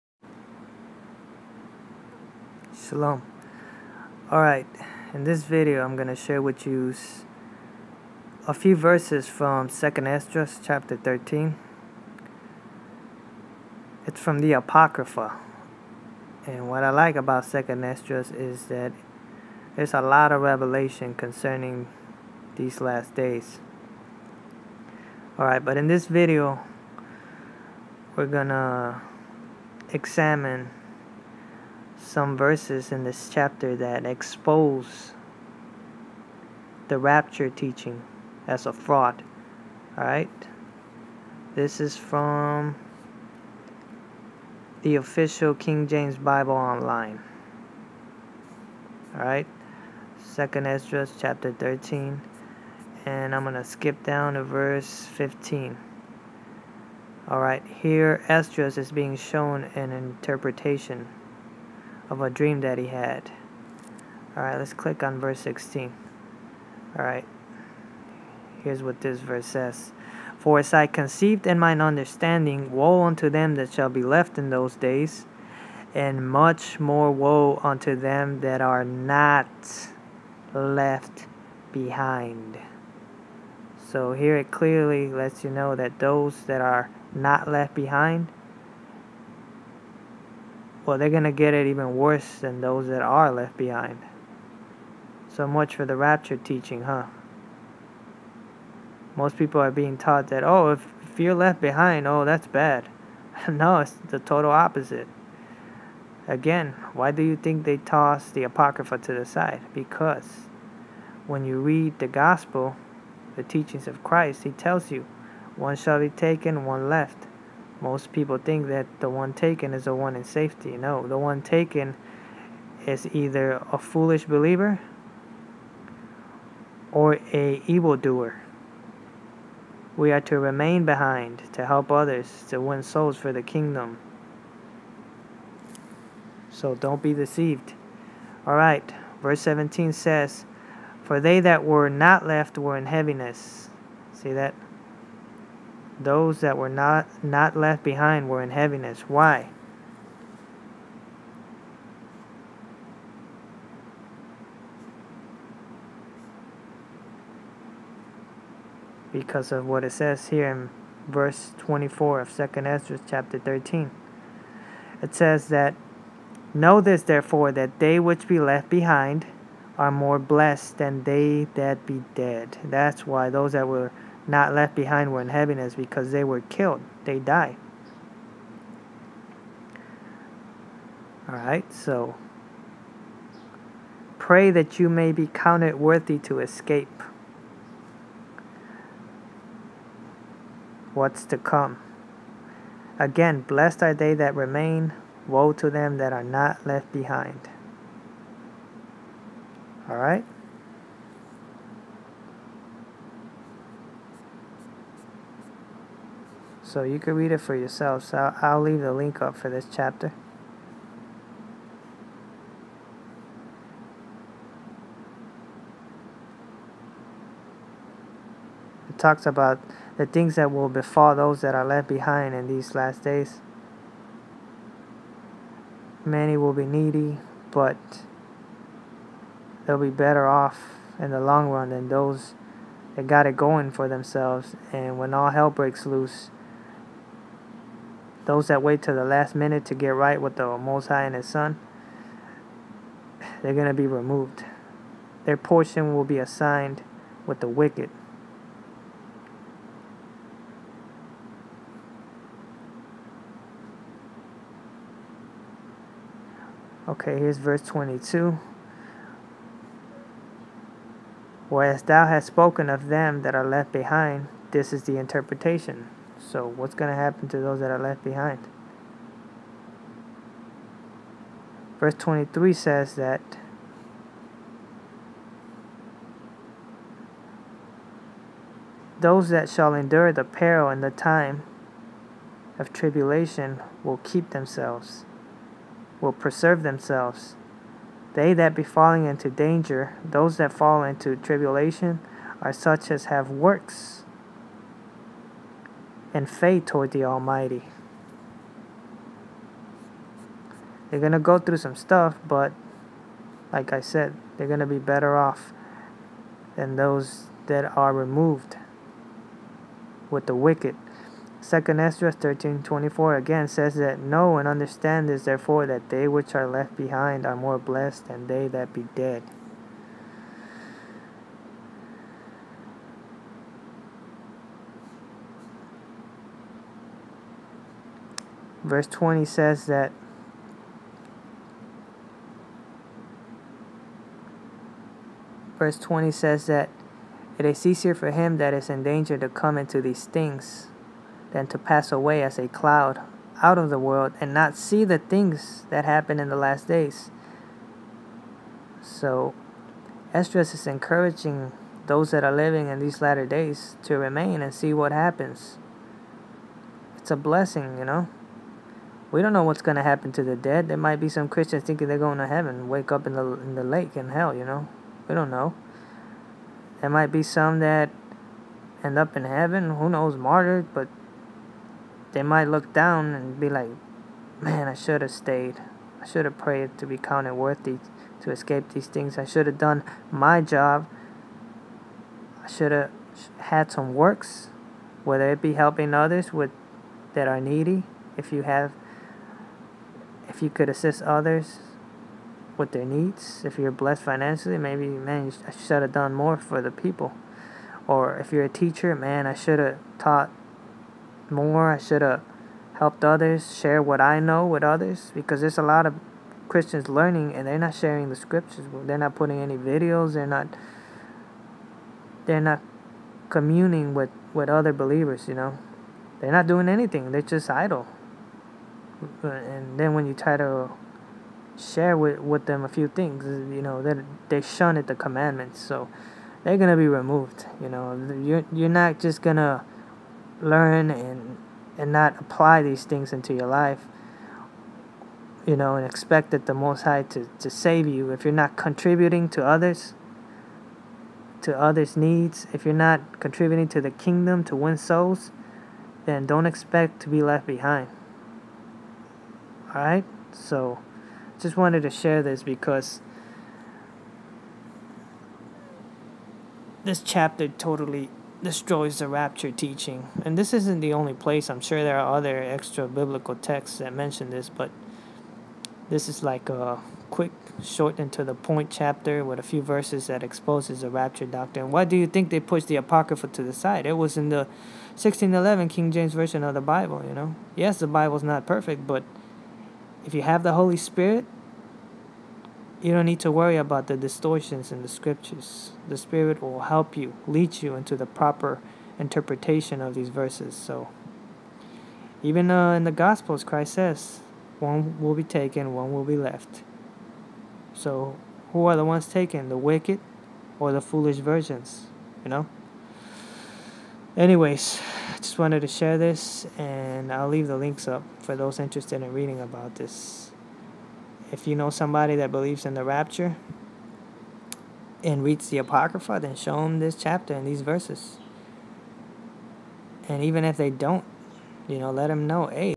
2nd Esdras destroys the rapture doctrine
The Rapture exposed by 2nd Esdras Ch.13 V.15-24, read for yourself